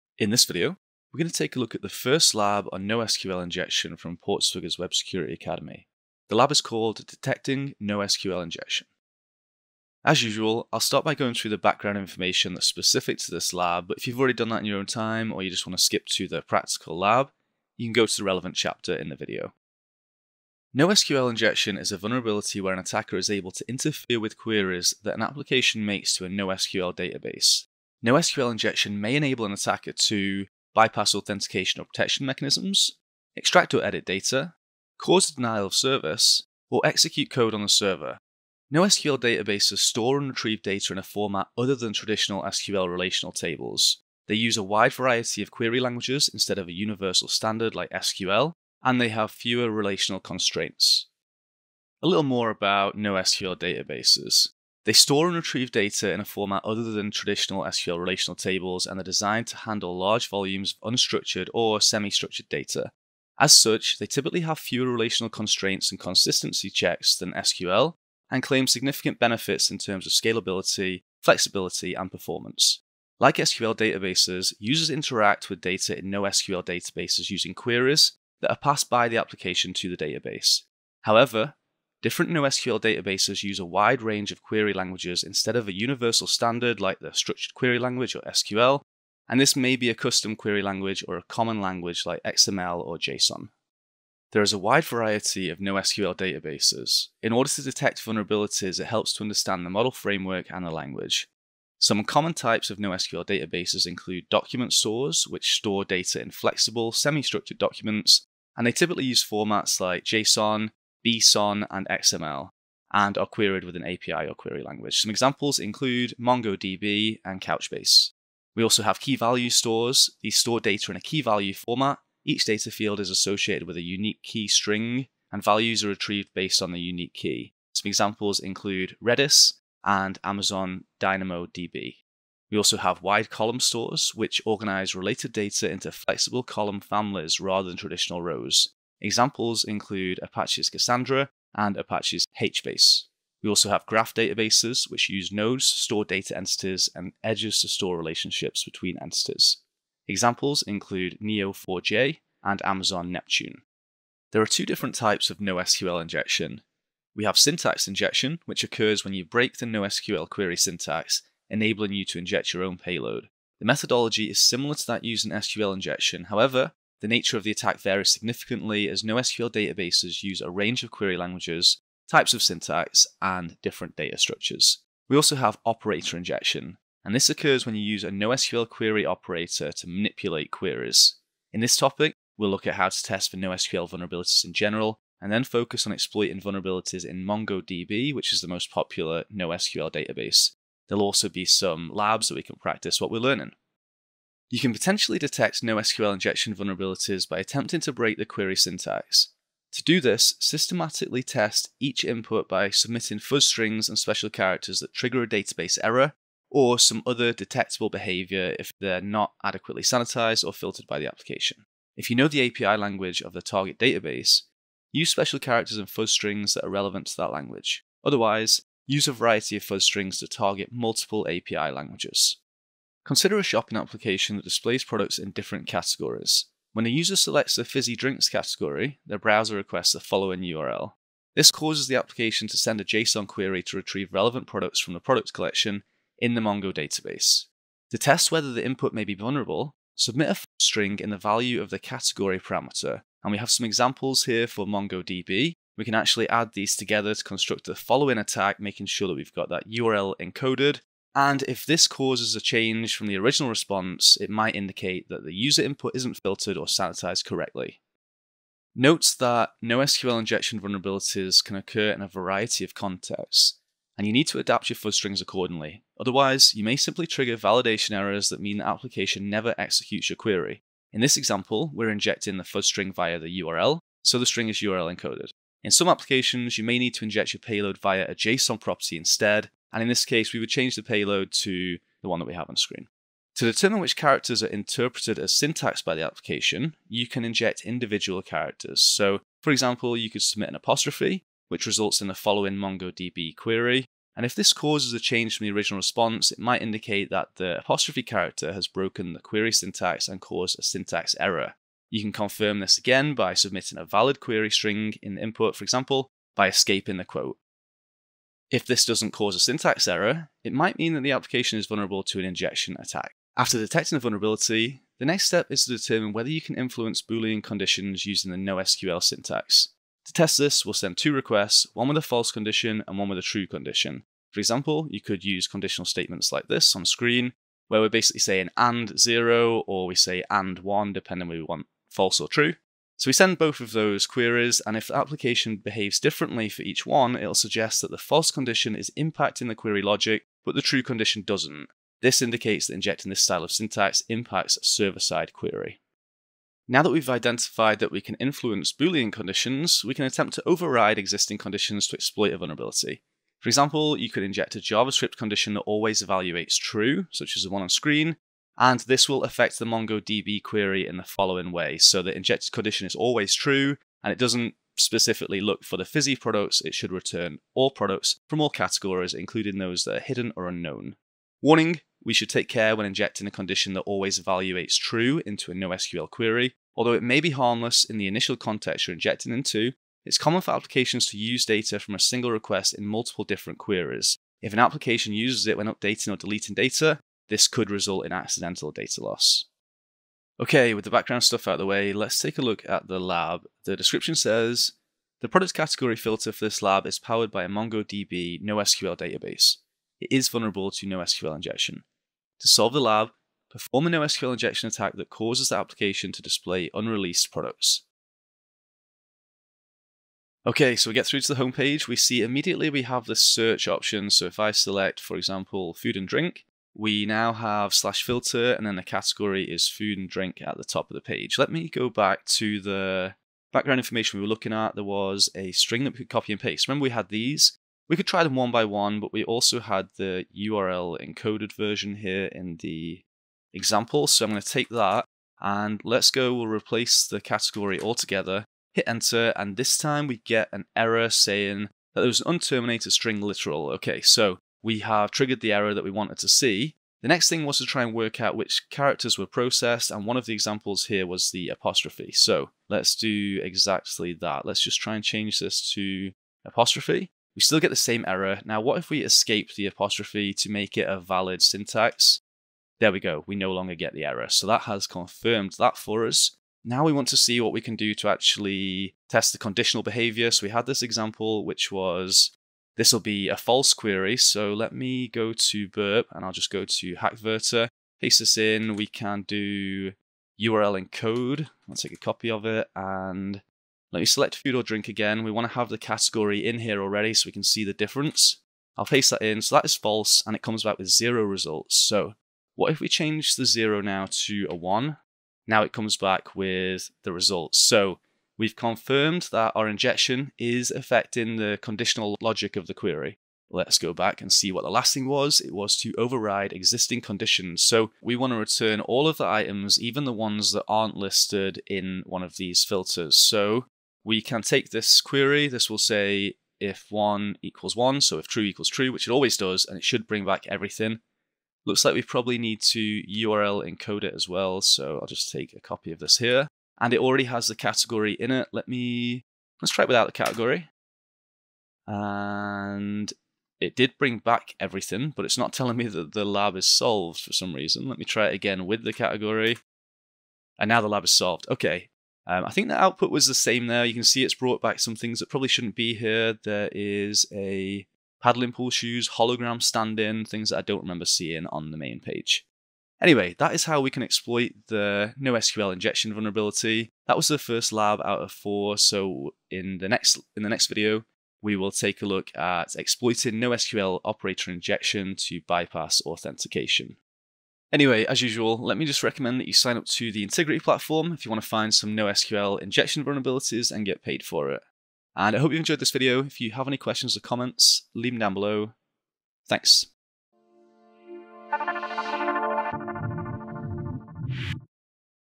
Detecting NoSQL Injection
👩‍🎓👨‍🎓 Learn about NoSQL injection attacks! The product category filter for this lab is powered by a MongoDB NoSQL database. It is vulnerable to NoSQL injection. To solve the lab, we'll perform a NoSQL injection attack that causes the application to display unreleased products.

If you're struggling with the concepts covered in this lab, please review

👕 Do you want some Intigriti Swag?

Overview:
0:00 Intro
0:20 NoSQL injection
0:55 NoSQL databases
1:34 NoSQL database models
2:34 Types of NoSQL injection
3:14 NoSQL syntax injection
6:30 Lab: Detecting NoSQL injection
9:13 Conclusion